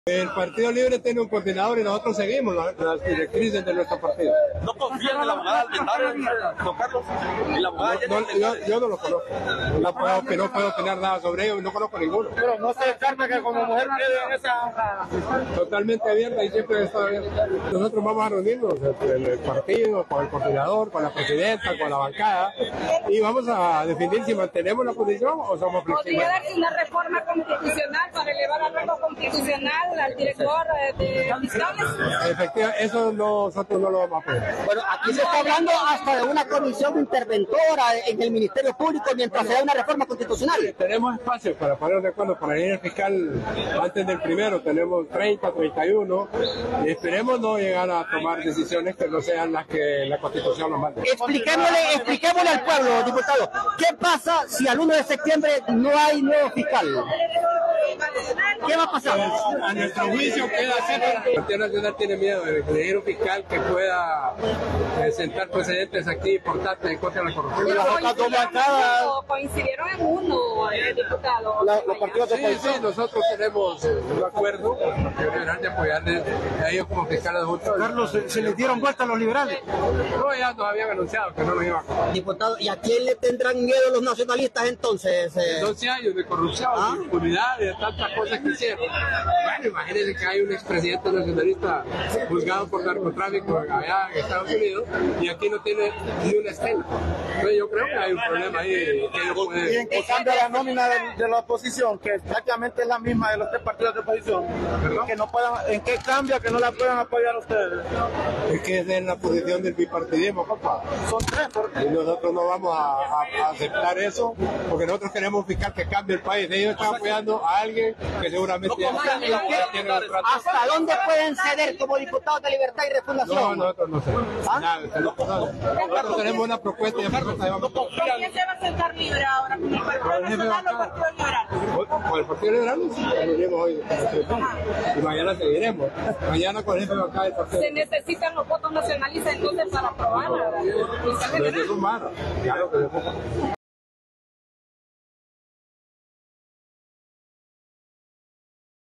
Carlos Zelaya: Esperemos no llegar a decisiones fuera de la Constitución en elección del Fiscal
El secretario del Congreso Nacional (CN), Carlos Zelaya, expresó que esperan no llegar a decisiones fuera de la Constitución de la República, si no se logran consensos con las diferentes bancadas para la elección del Fiscal General y Adjunto.